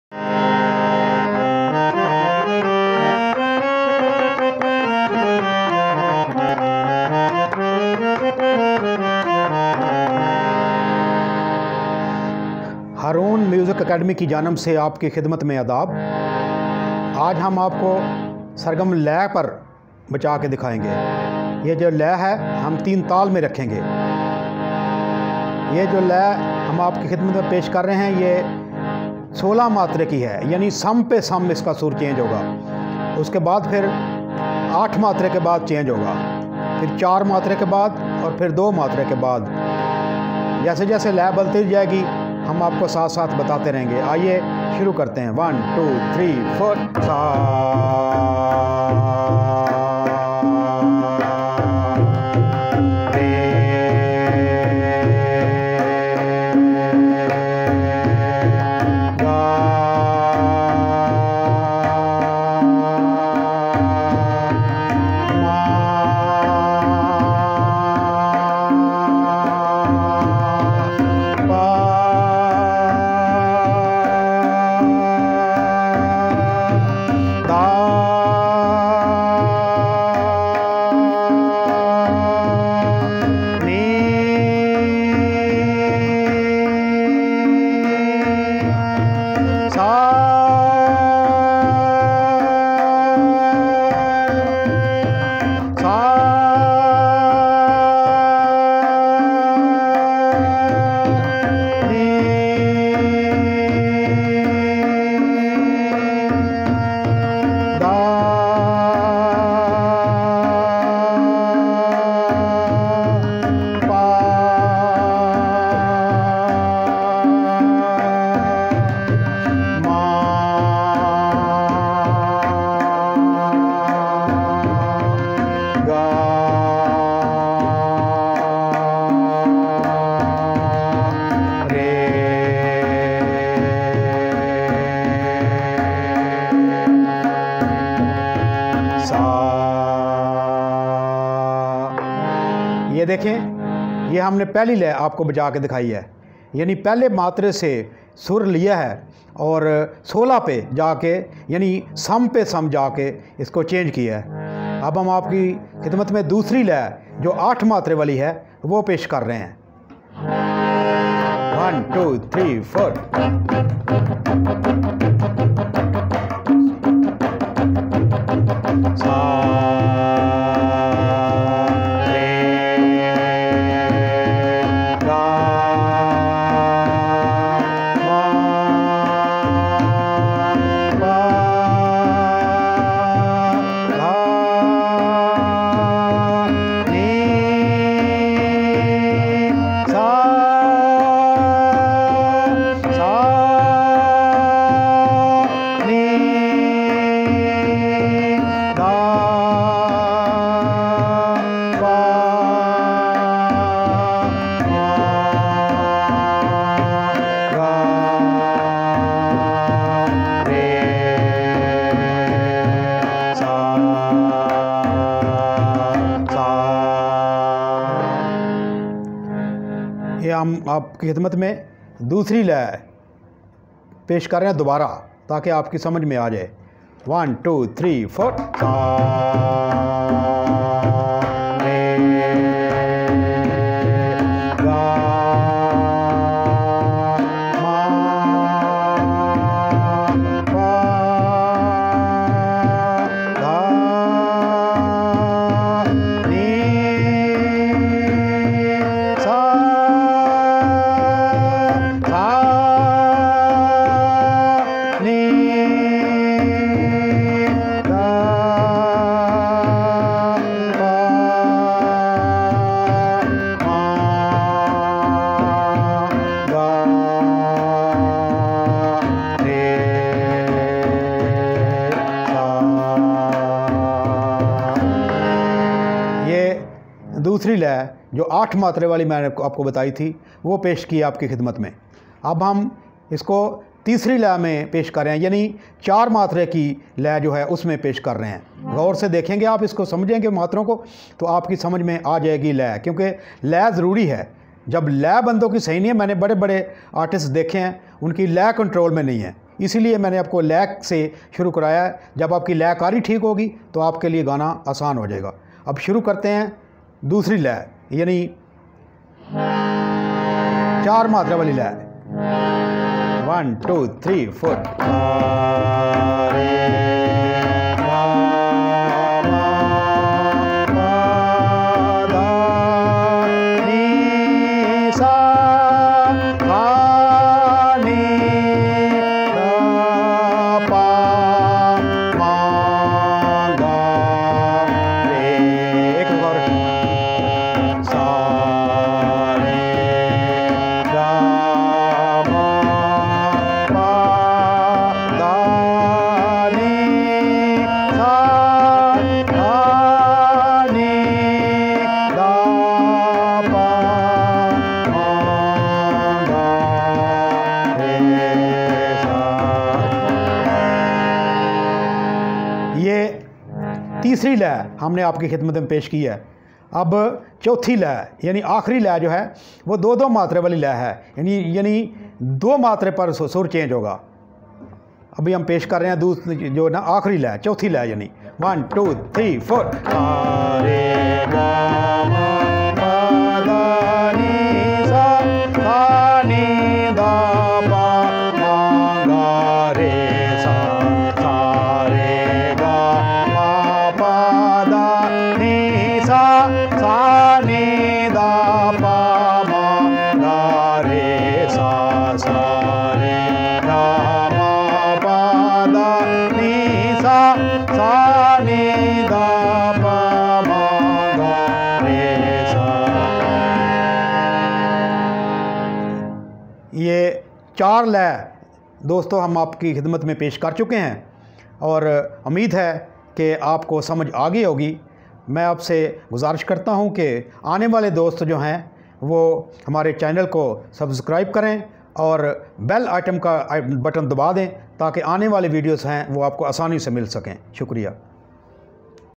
Learn- lesson 1 Raag bharivai sargam with tabla lay in hindi urdu/ Haroon Music Academy
haroon music academy
is video my ham ap ko raag bharivai ki sargarm table kay sath bajna sekhye gay
sargarm
Saa Ree Gaa Maa Paa Da Ni SA
Saa Ni Daa Paa Maa Gaa Ree Sa